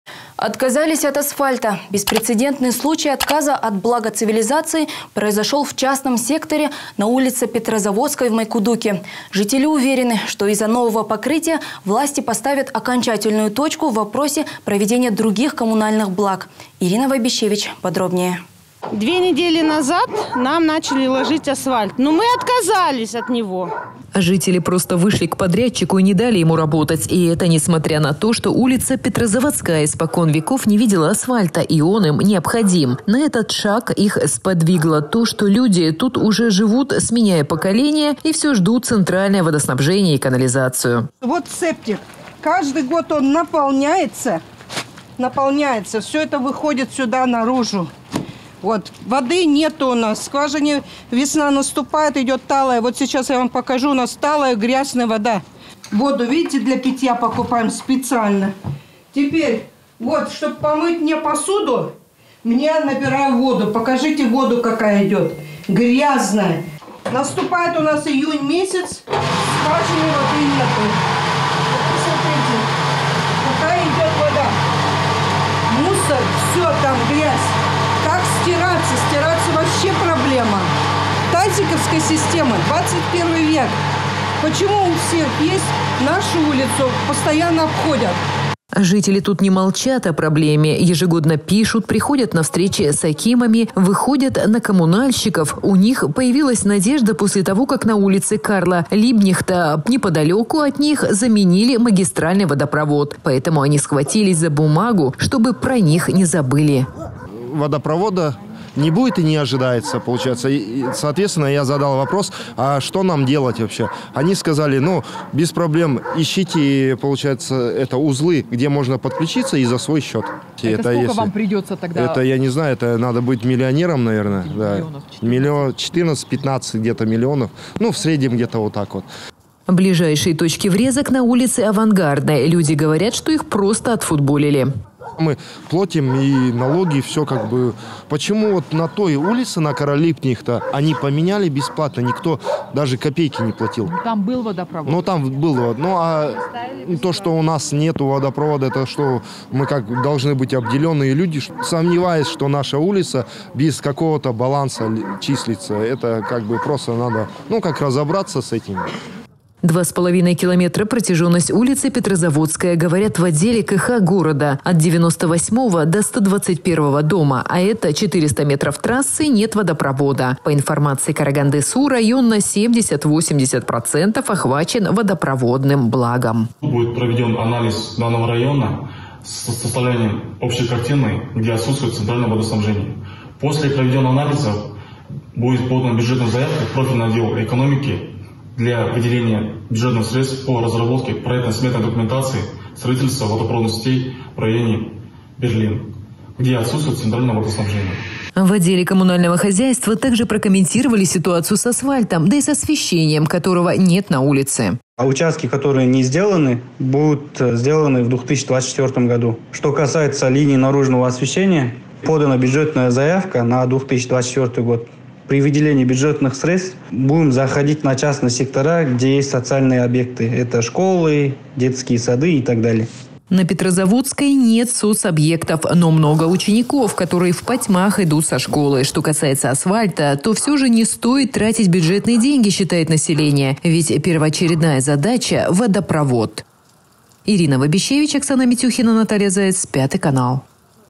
Отказались от асфальта.
Беспрецедентный случай отказа от блага цивилизации произошёл в частном секторе на улице Петрозаводской в Майкудуке. Жители уверены, что из-за нового покрытия власти поставят окончательную точку в вопросе проведения других коммунальных благ. Ирина Вабищевич подробнее.